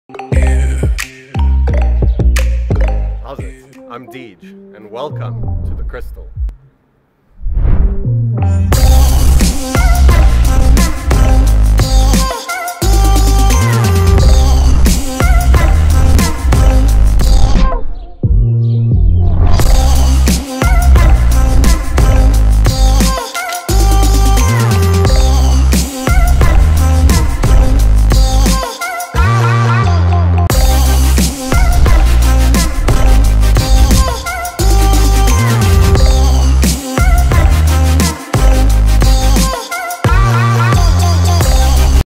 THE CRYSTAL
This exquisite apartment, located in sought-after The Crystal, offers luxury and exclusivity and boasts magnificent views of the ocean and mountains.

Oozing with style and charm, this modern top-floor apartment includes three large bedrooms - all having their own full bathrooms, a guest loo, a naturally lit open-plan kitchen with separate scullery, a double volume lounge and dining area that leads onto a large balcony equipped with rolling shutters, two secure, dedicated underground parking bays, magnificent views of the ocean and Lions Head from the front balcony and the Twelve Apostles from the upstairs balcony as well as a large communal swimming pool.

If luxury and exclusivity is what you're after, then look no further!